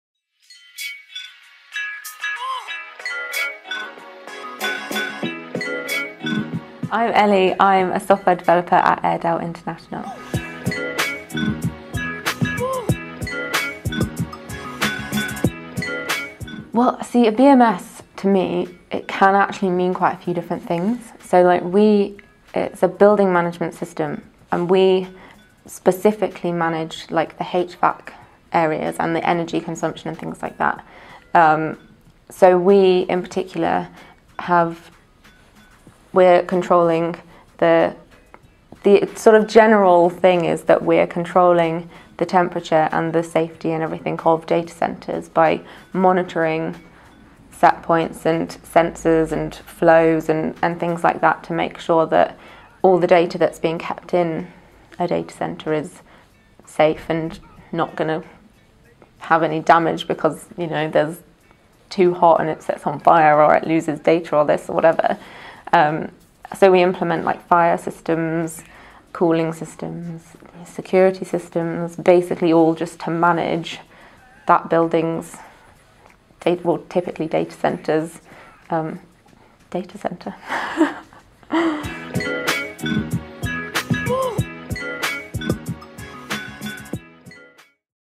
What is a Building Management System?
In this video, Eleanor Hall describes what a BMS system is.